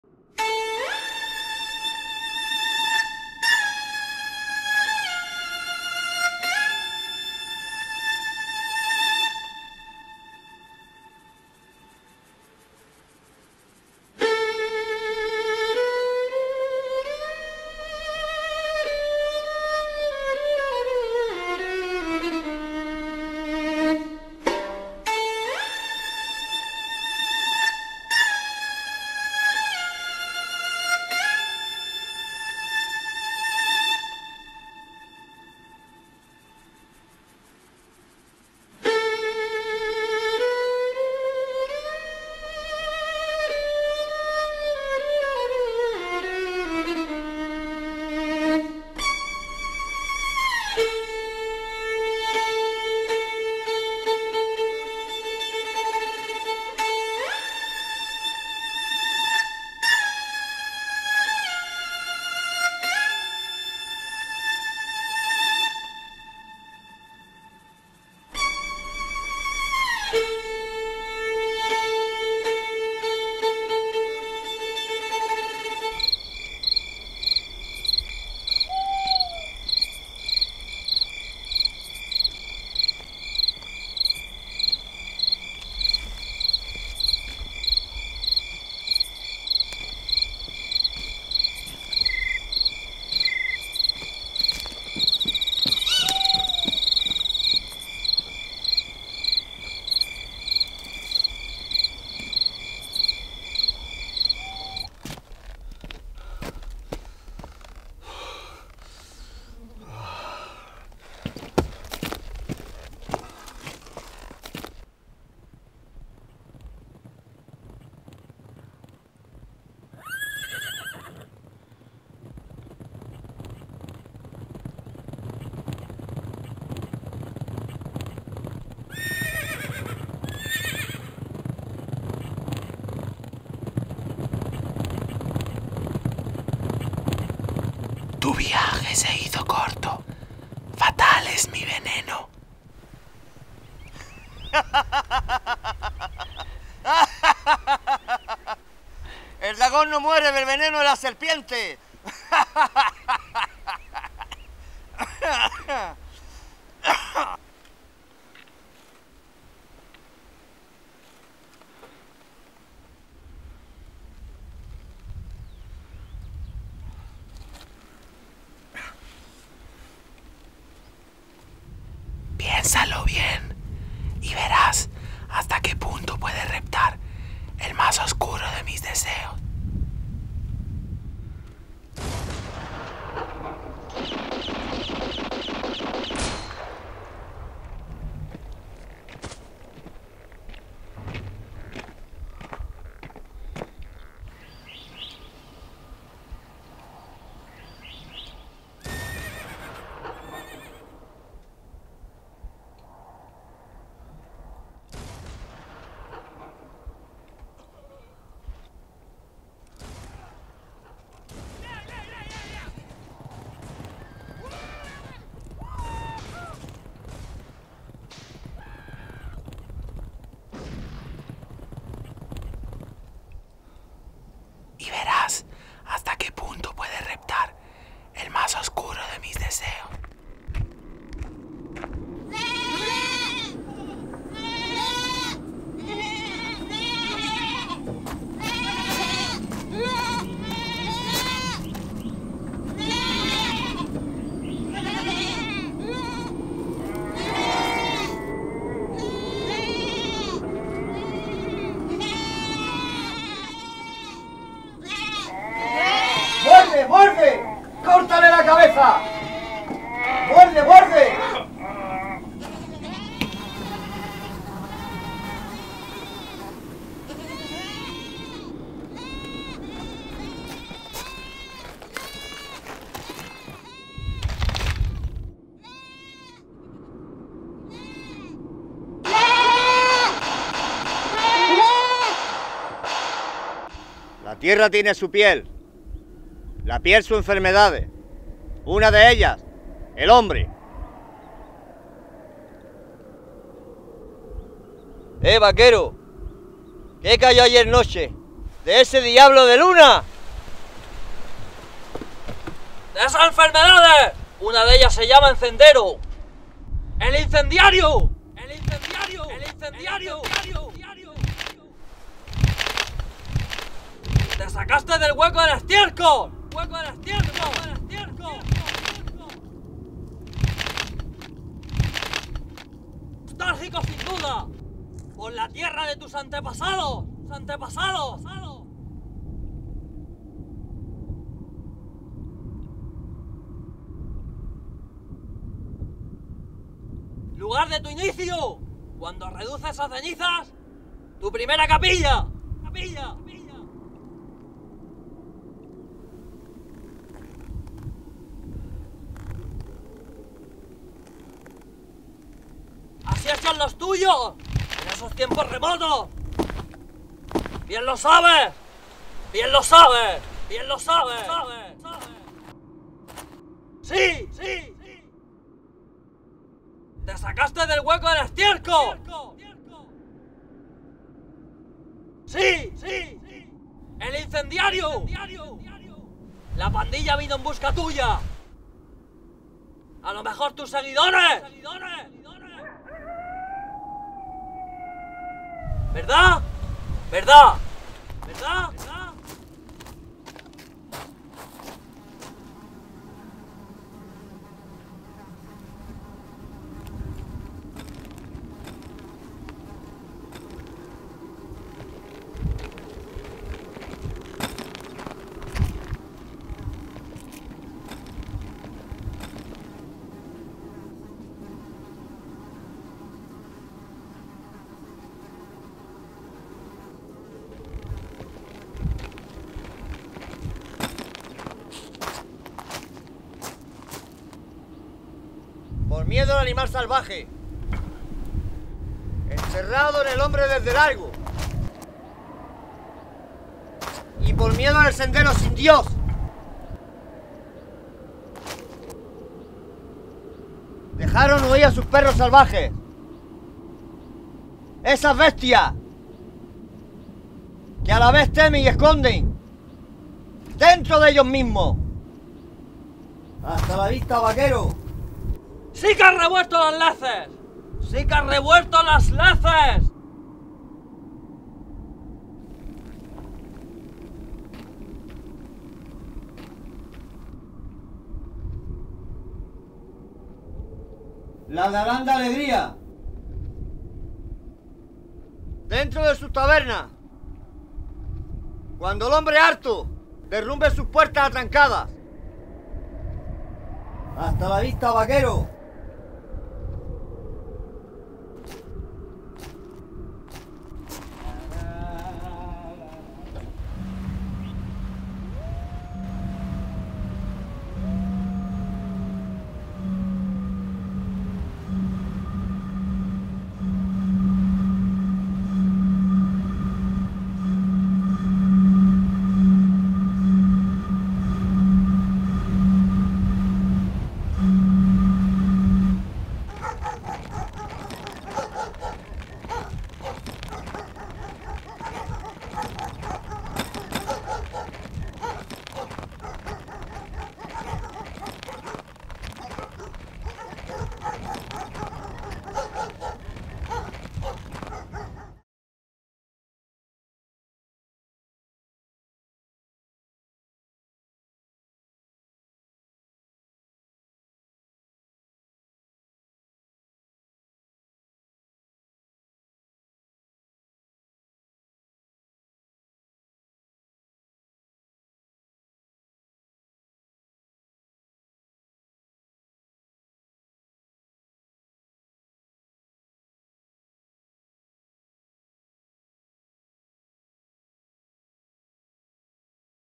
LA VOLUNTAD MAS OSCURA
The eleventh western style episode in the ENCENDERO EL TIZONAZO series based on 'Thus spoke Zarathustra' by Friedrich Nietzsche; shot around the Tabernas desert, Almeria, Spain summer 2015; directed by Anders Gullberg; featuring Little Bear Antonio Ramirez Segura and Claudio Lopes Guirado.